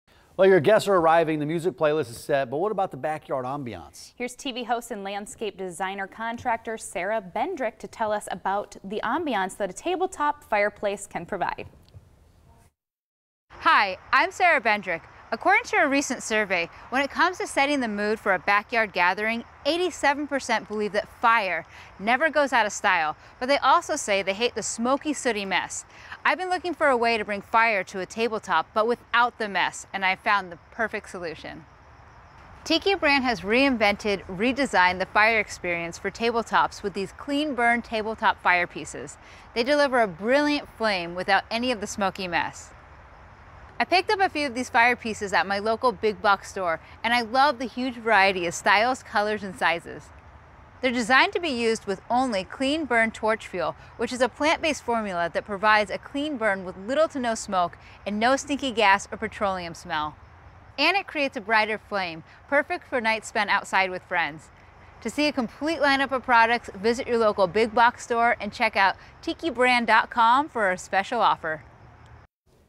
Tabletop Fireplaces
Your guests are arriving, the music playlist is set, but what about the backyard ambiance? Here’s TV Host and Landscape Designer/Contractor, Sara Bendrick, to talk about the ambiance that a tabletop firepiece can provide.